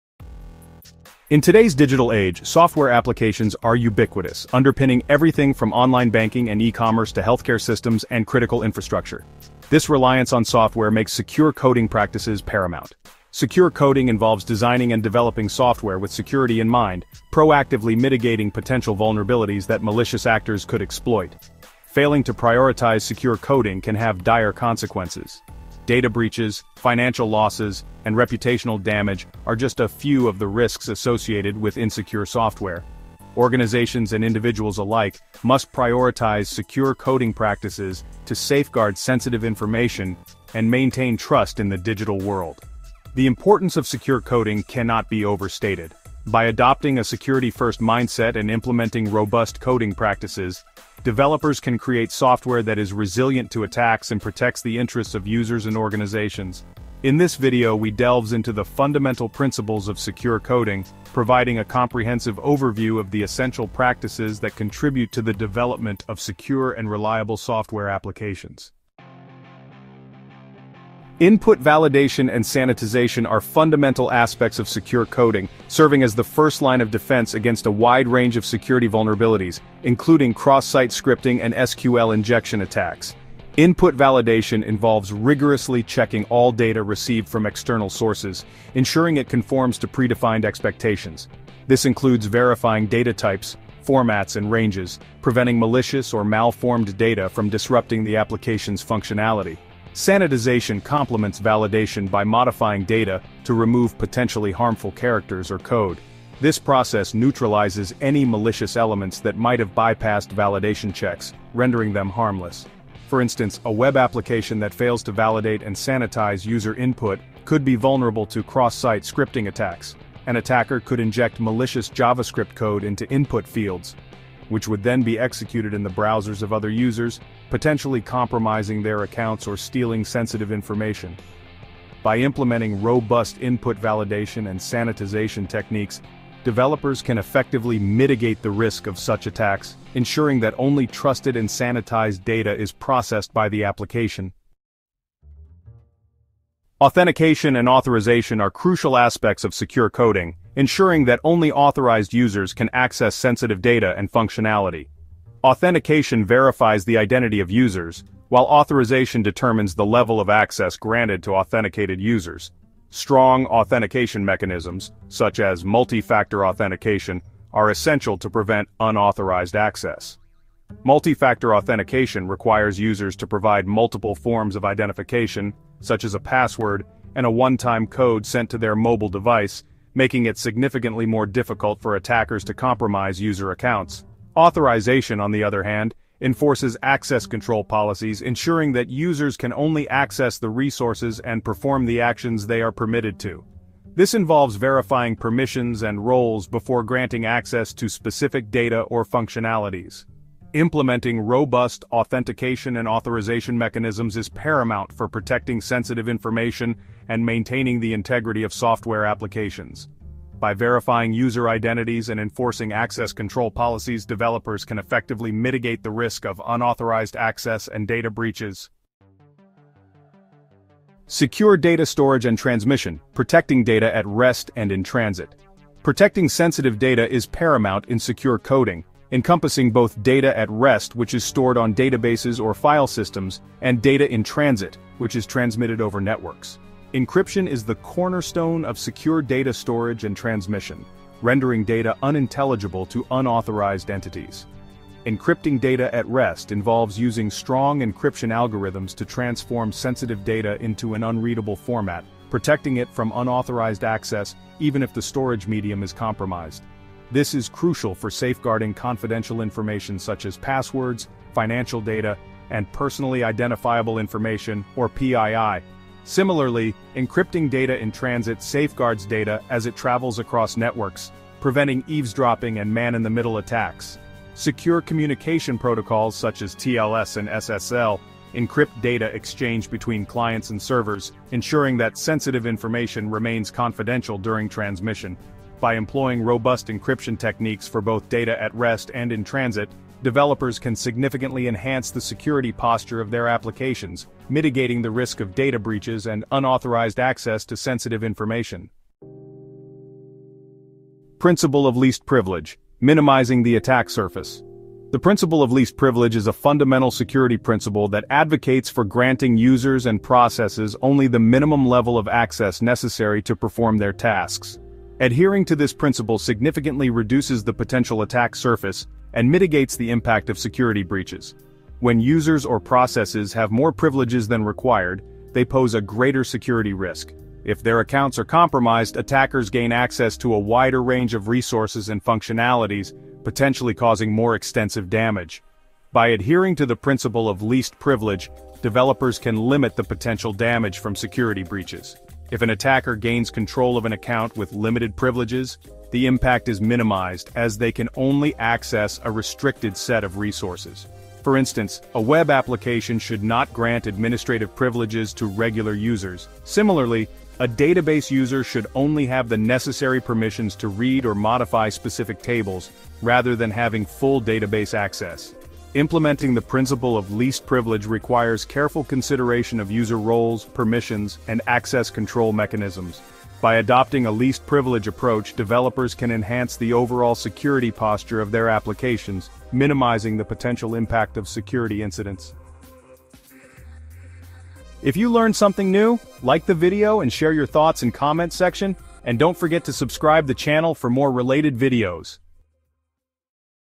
Exploring the Fundamentals of Secure Coding Practices | MS Learning
In this comprehensive video, "Exploring the Fundamentals of Secure Coding Practices," we dive deep into essential best practices and principles that every developer should know to keep their code secure from vulnerabilities. Whether you’re a seasoned programmer or just starting out, understanding secure coding is crucial in today’s digital landscape. We’ll cover topics such as input validation, proper error handling, code reviews, and the importance of security frameworks. With clear explanations and practical examples, you'll gain the knowledge needed to write secure code and protect your applications from potential threats. Join us on this vital journey towards better coding standards!

OUTLINE:

00:00:00 The Importance of Secure Coding
00:01:08 First Line of Defense
00:02:24 Controlling Access to Secure Data
00:03:38 Protecting Data at Rest and in Transit
00:04:58 Minimizing the Attack Surface
00:06:22 Outro